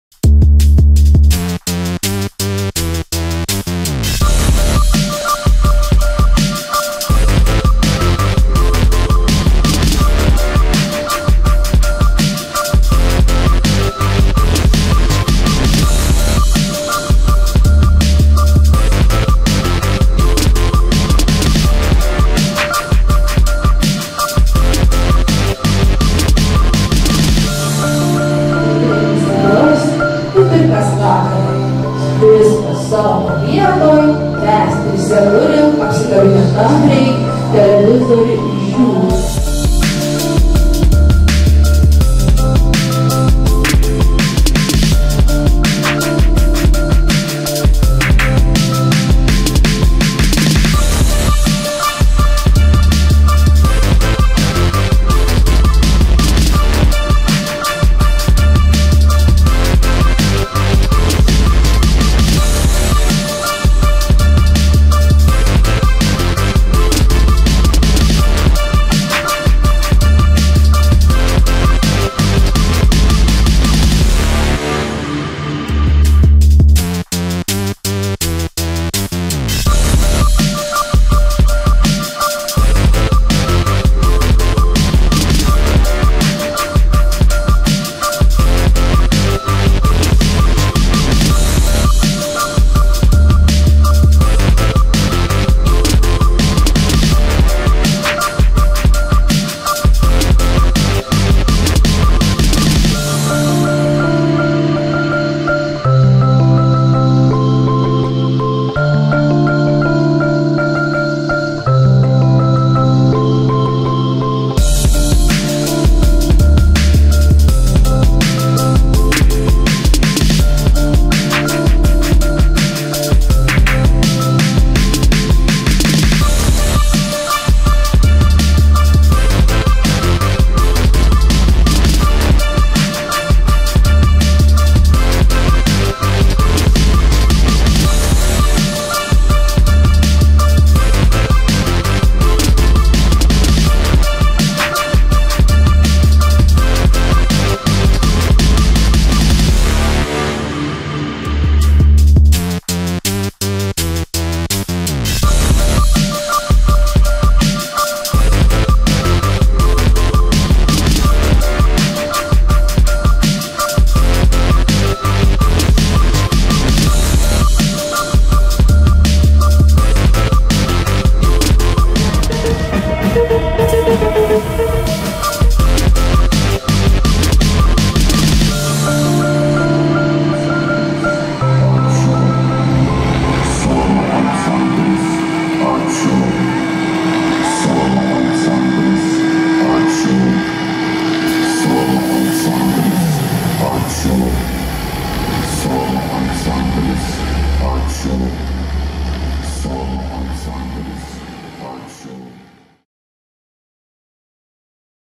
Festivalio "Kvadratu 2019" klipas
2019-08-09 Tauragės pilyje muziejus surengė audiovizualinių menų festivalį „Kvadratu“, kuris sulaukė didelio pasisekimo ir teigiamų įvertinimų. Siūlome pažvelgti į gražiausias festivalio akimirkas.